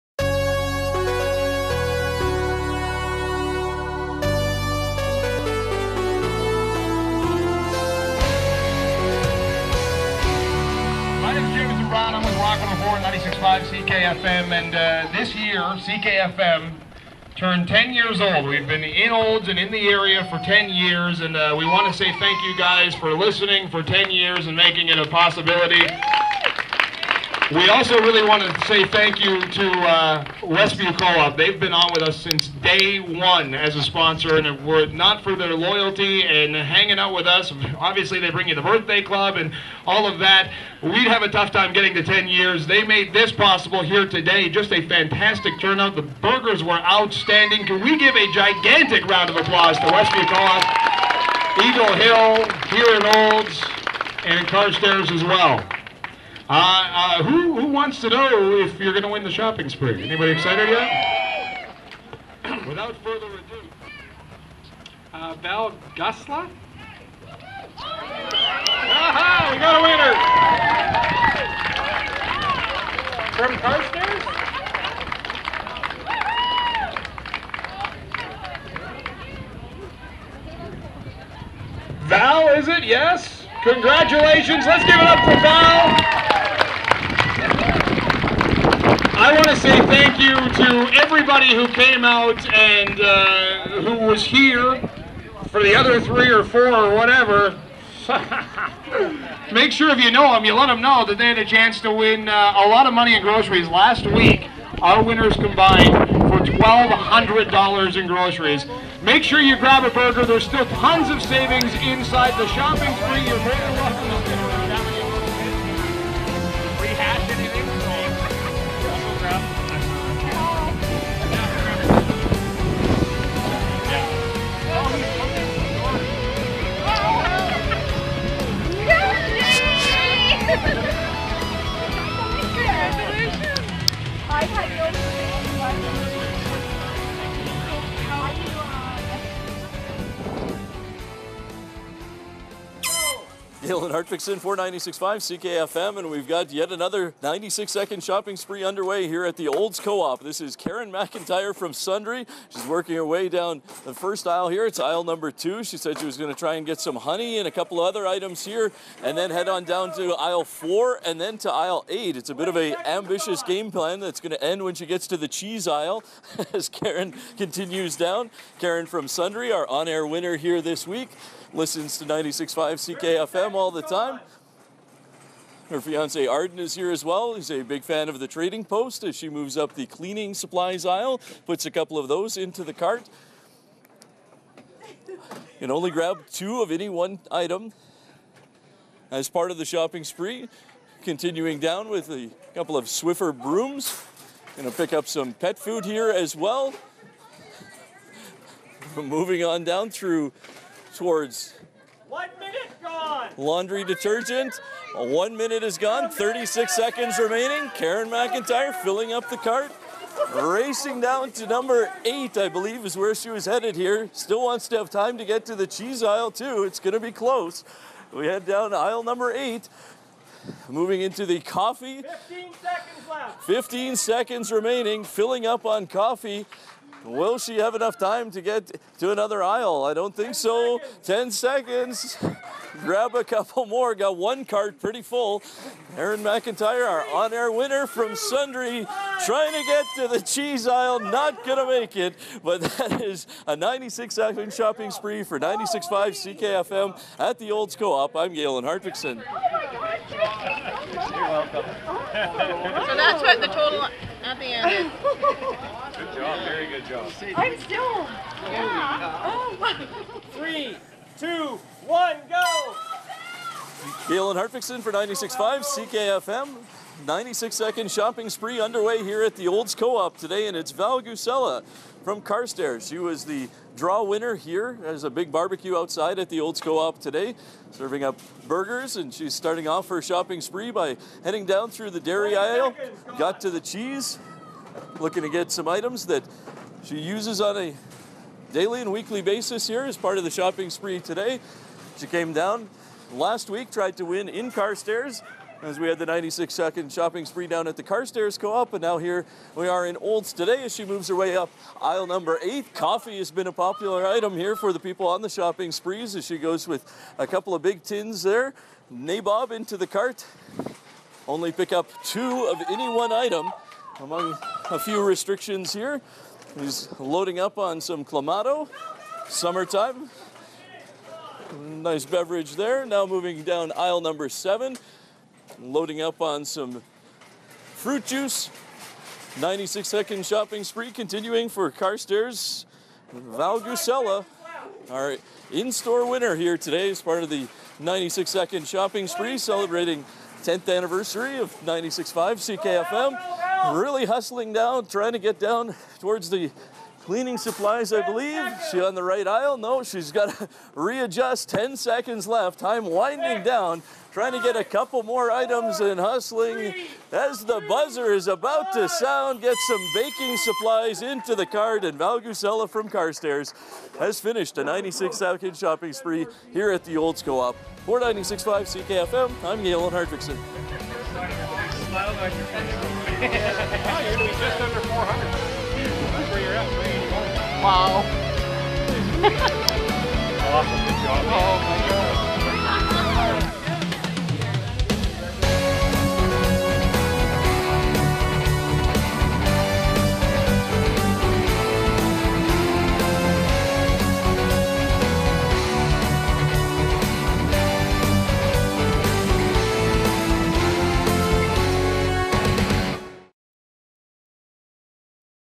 The Great 96.5 Second Shopping Spree #2
2 Winners each with 96.5 Seconds to fill their Carts with Free Groceries at Westview CO-OP in Olds, AB as part of the 96.5 CKfm 10th Anniversary Celebrations!